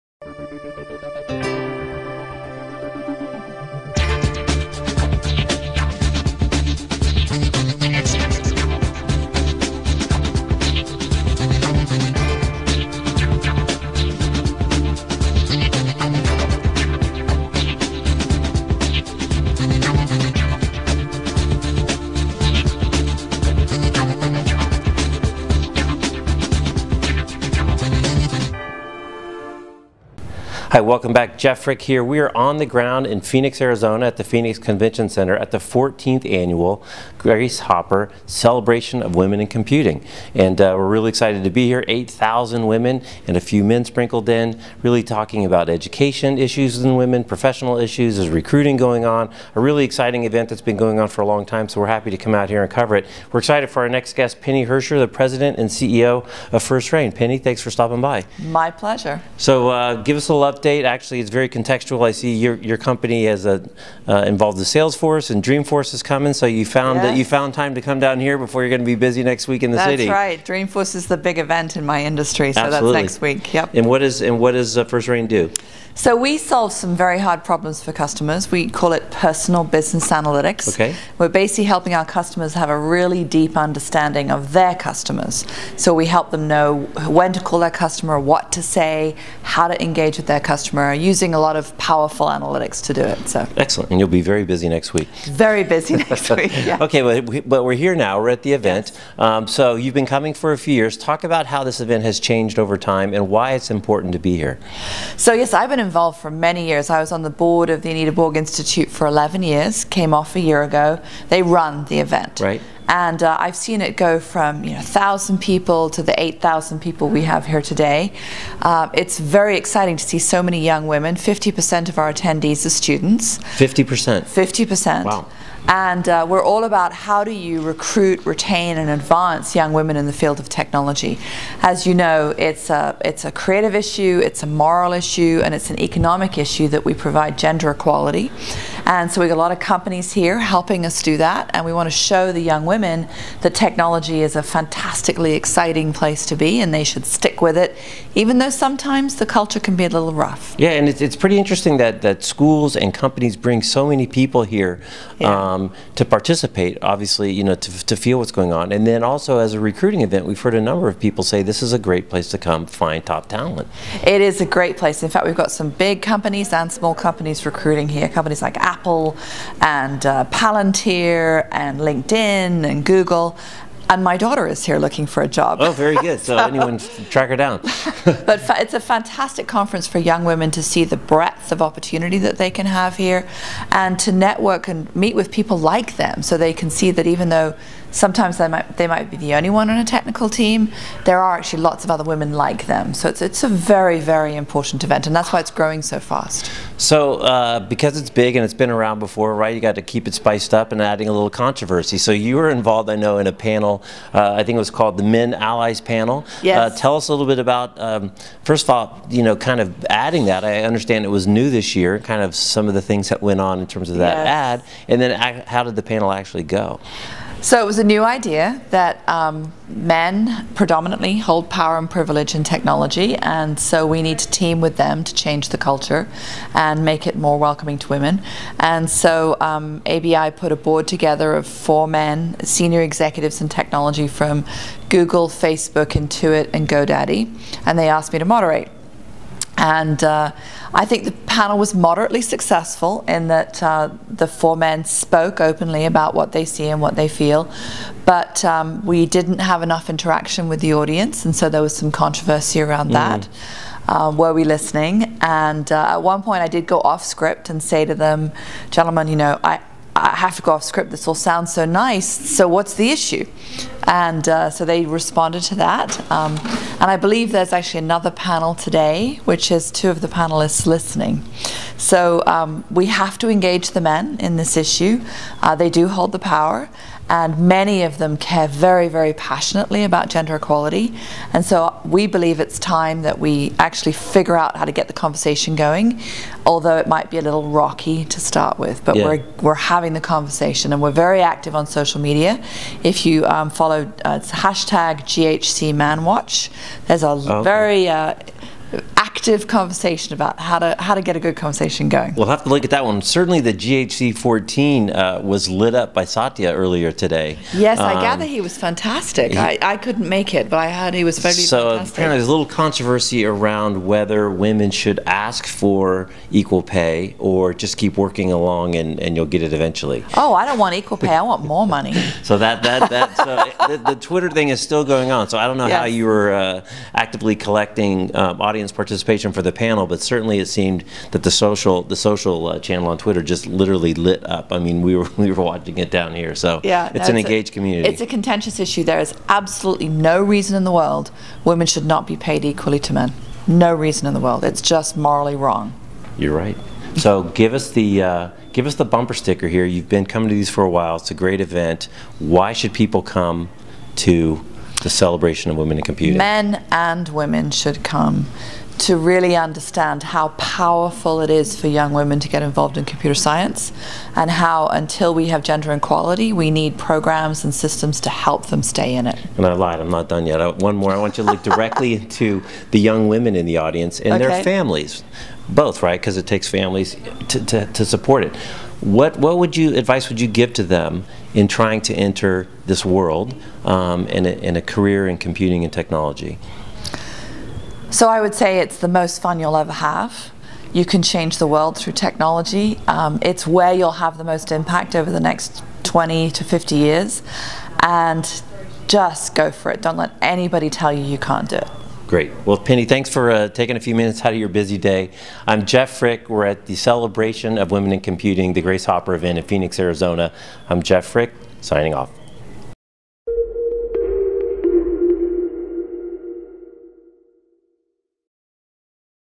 Penny Herscher, First Rain | Grace Hopper Celebration of Women in Computing 2014
Penny Herscher, FirstRain, at the Grace Hopper Celebration of Women in Computing (2014) with Jeff Frick